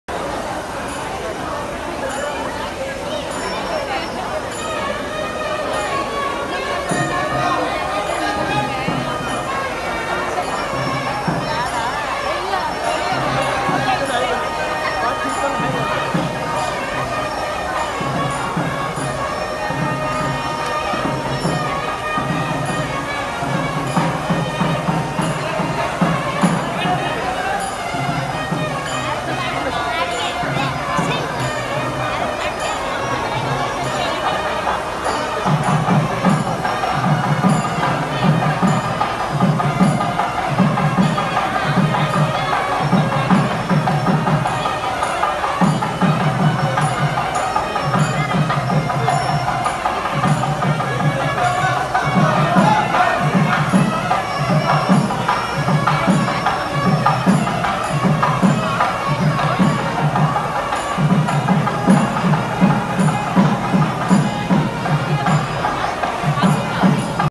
Vijay attended Jashwanth Kanna Priyanka's wedding
Actor Vijay attended the wedding of Jashwanth Kanna s/o Ramesh Kanna at Shree varaaham hall, koyembedu, Chennai.